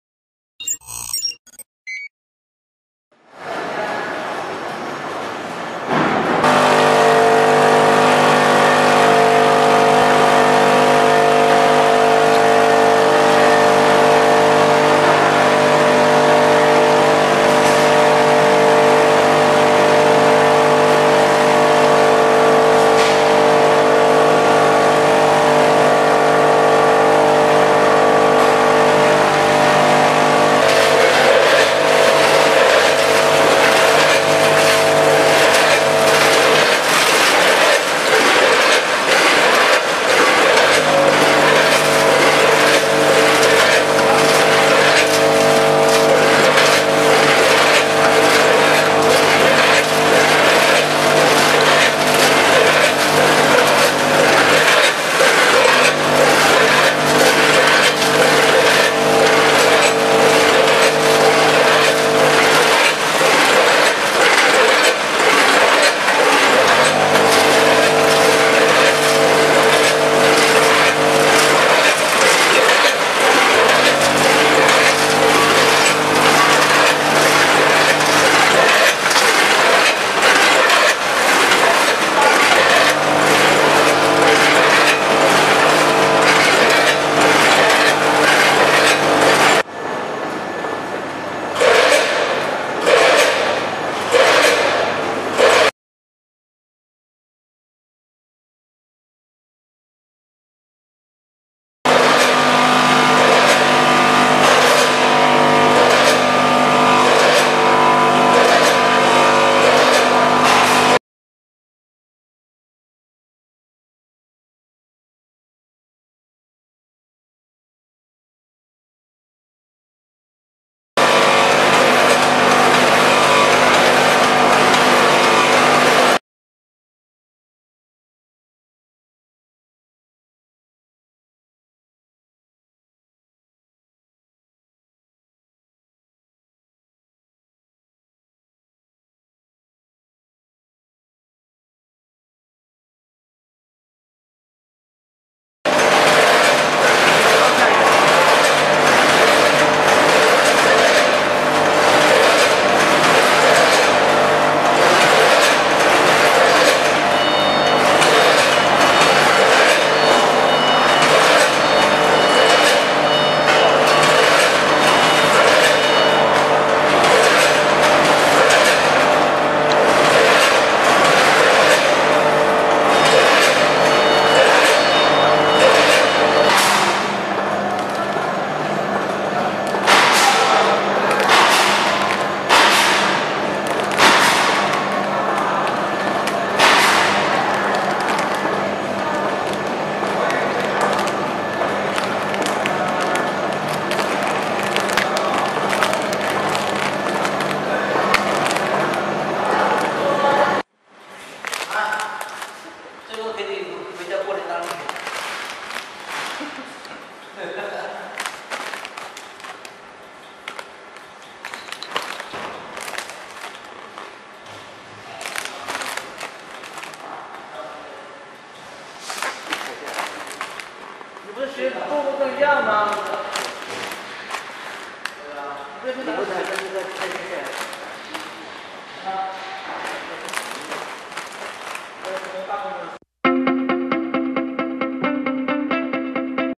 Automatic VFFS Machine for Chips with High Speed
Features:
1.New design with perfect fashion, more reasonable structure, more
advanced technology.
2.Imported PLC control system, colorful touch screen, easy operation, Intuitionistic
and efficient.
3.Standard positioning with imported servo film transporting system and mark sensor, super-excellent capability for complete machine, perfect packaging.
4.With auto warning protection function to minimize loss.